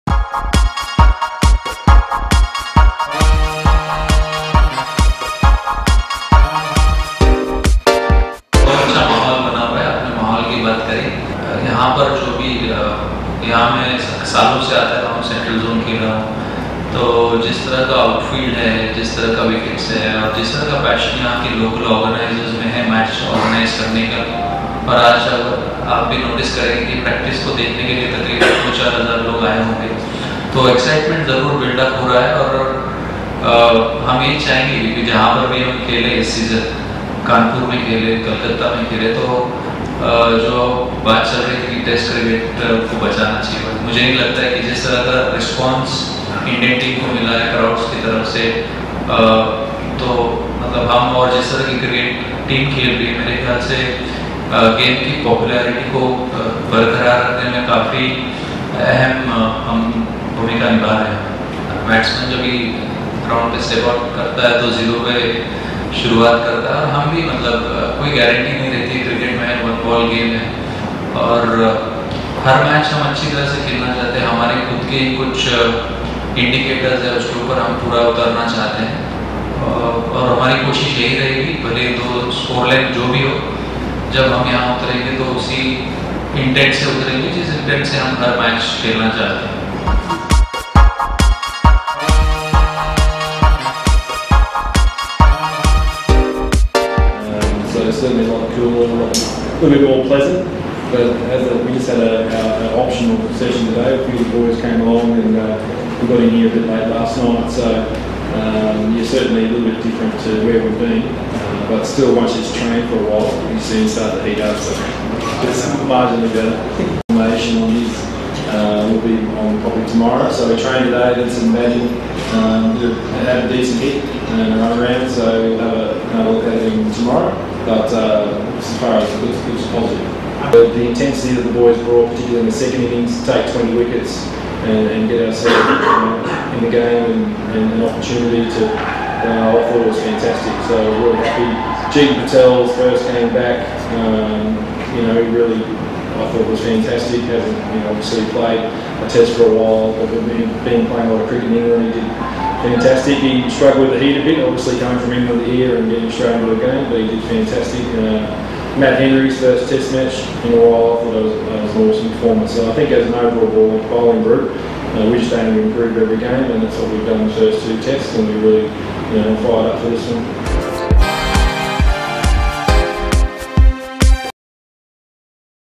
इंदौर के लोगों में क्रिकेट का जुनून
इंदौर में पहली बार भारत और न्यूजीलैंड टेस्ट मैच 8 से 12 अक्टूबर तक
* मध्यप्रदेश क्रिकेट एसोसिएशन के होलकर स्टेडियम में टेस्ट मैच को लेकर जबरदस्त माहौल
* दोपहर में टीम अभ्यास सत्र में इंदौरी दर्शकों ने खिलाड़ियों का बढ़ाया हौसला
* टेस्ट क्रिकेट में दर्शकों से मिले प्रोत्सासन से भारत के सहायक कोच बांगर खुश
* संजय बांगर ने टेस्ट मैच को लेकर मध्यप्रदेश क्रिकेट एसोसिशन की प्रशंसा की
* न्यूजीलैंड टीम के गेंदबाजी कोच शेन जगनसन ने कहा, हम दोनों टेस्ट हारे जरूर लेकिन वे बेहद रोमांचक रहे